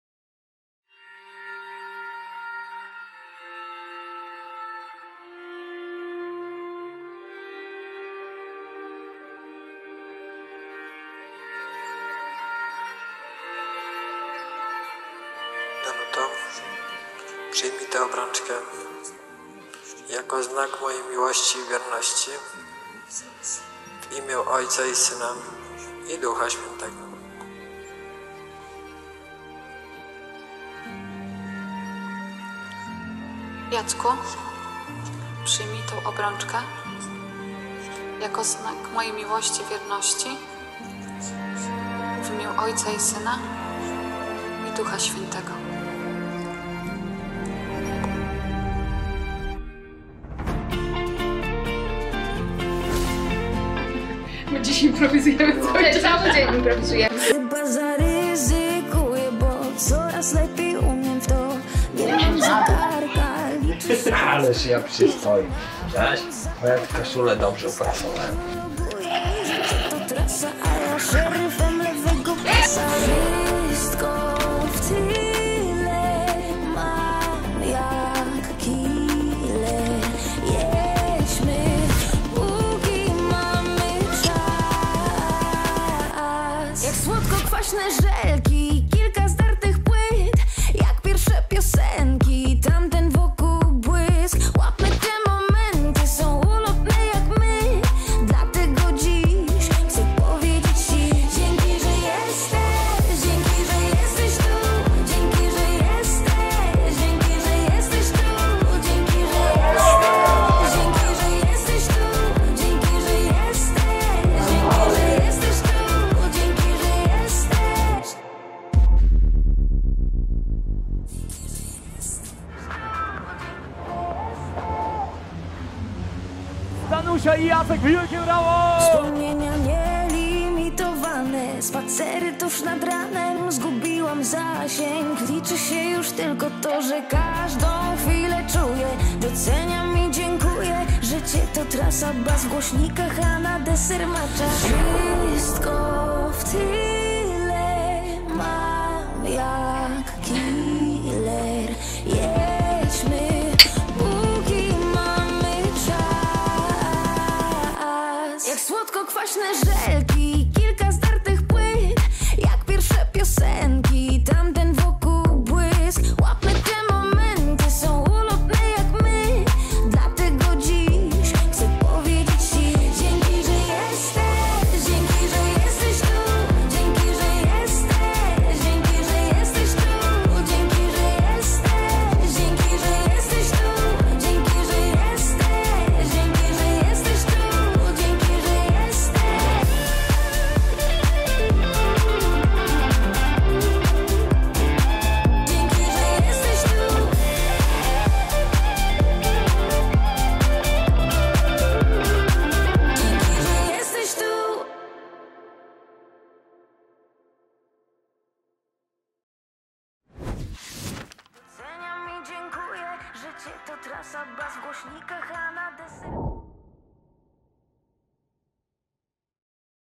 Danusia i Jacek | Dzięki, że jesteś tu! / Karczma Stary Spichlerz Ełk
Film zrealizowany przez Fototraper Films w składzie:

🎬 Operator : Krzysztof Połajkowski
🎬 📸 Operator/montaż : Rafał Nowosielski
🎤 DJ Mani
🏰 - Karczma Stary Spichlerz Ełk
⛪️ - Katedra pw. św. Wojciecha